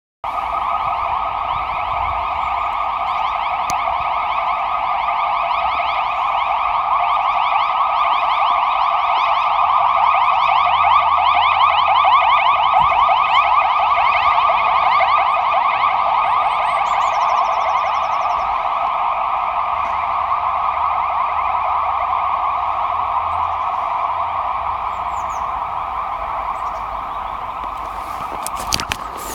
Karetka Vw Crafter (S) na sygnale
Karetka Volkswagen Crafter (S) z Falck Medycyna jedzie na sygnale do szpitala w Kędzierzynie-Koźlu.